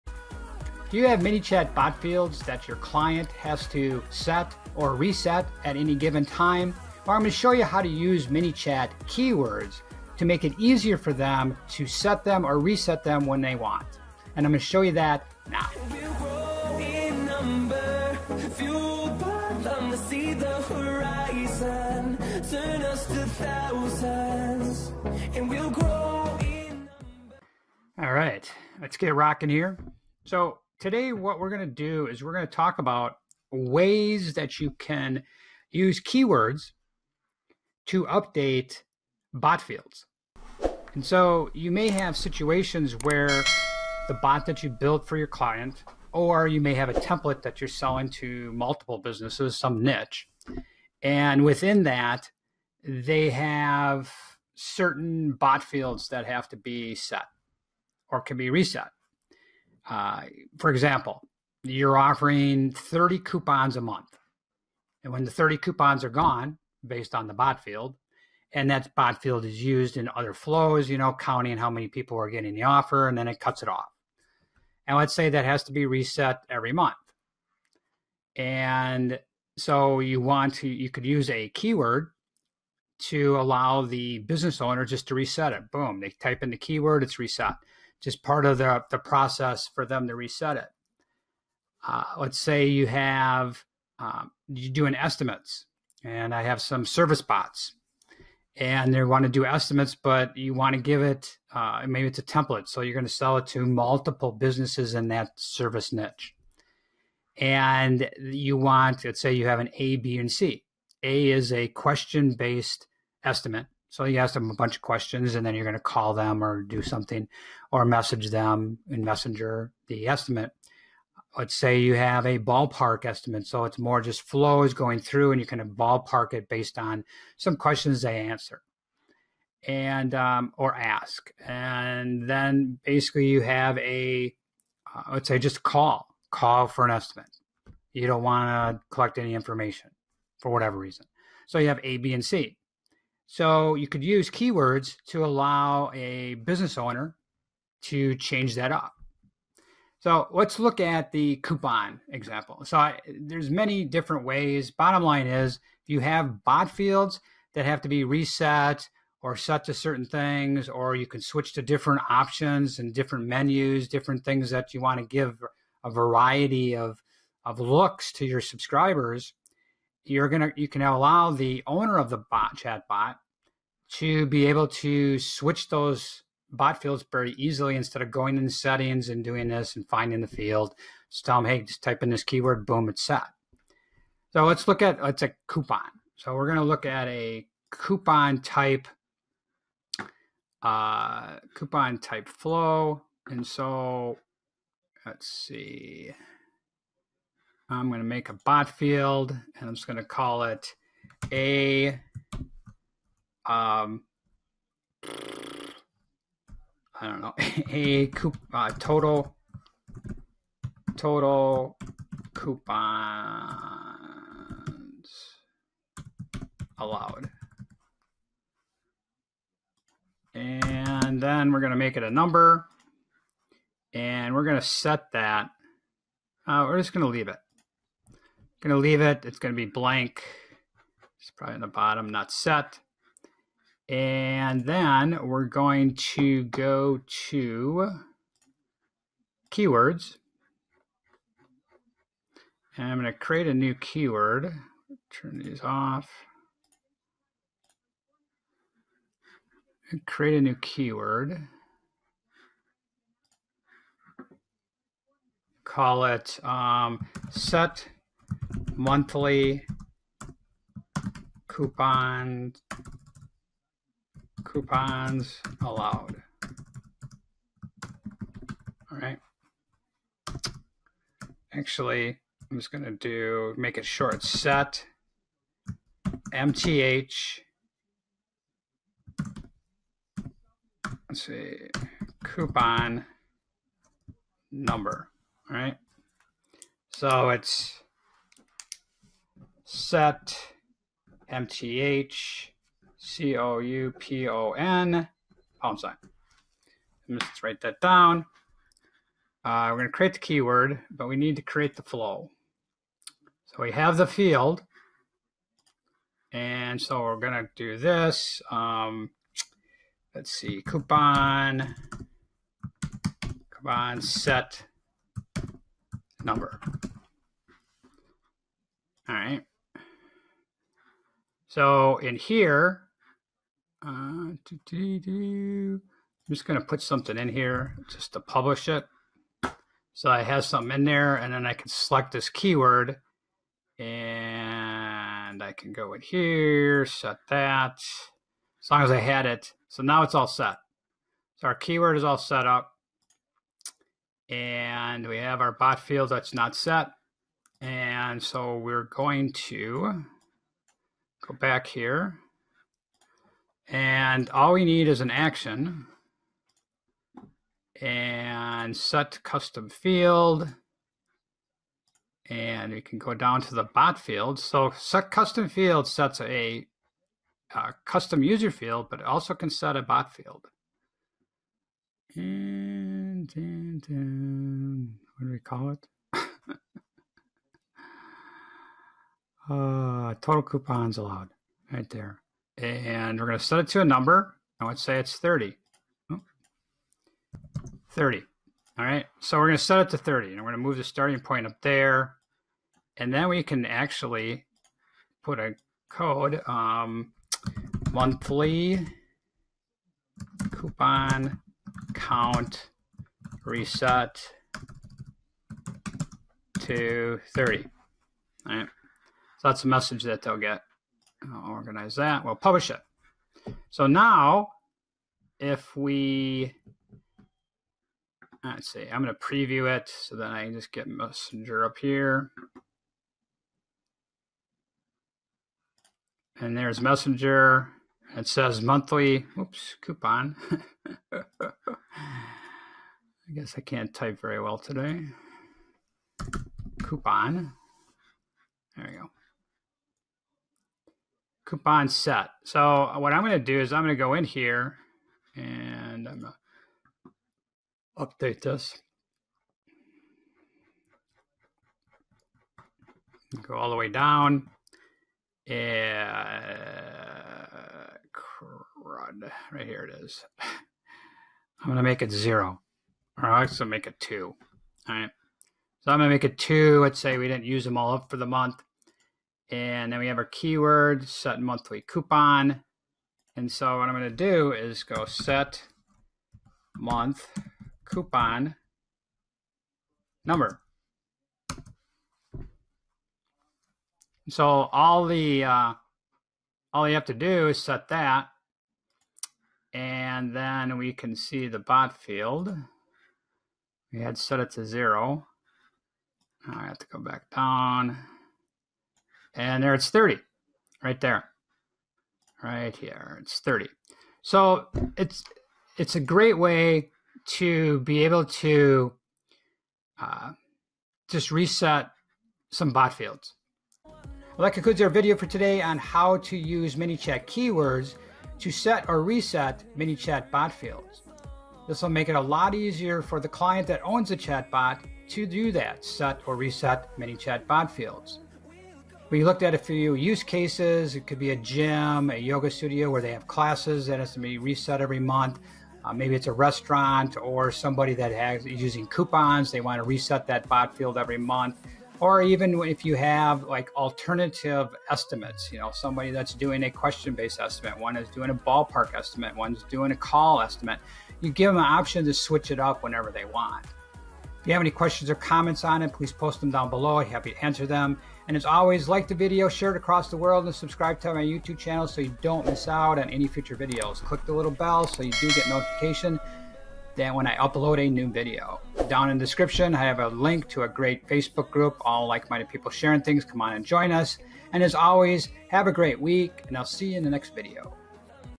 How to set ManyChat Bot Fields using ManyChat Keywords
How to set ManyChat Bot Fields using ManyChat Keywords.  A simple way to make it easy for the chatbot owner to set ManyChat Bot Fields.

Watch this ManyChat Tutorial video 🎥 if you want to learn:
- How to setup a ManyChat Bot Field.
- How to setup the ManyChat Flow using a ManyChat Action.
- How to setup the ManyChat Keyword to fire the Flow off.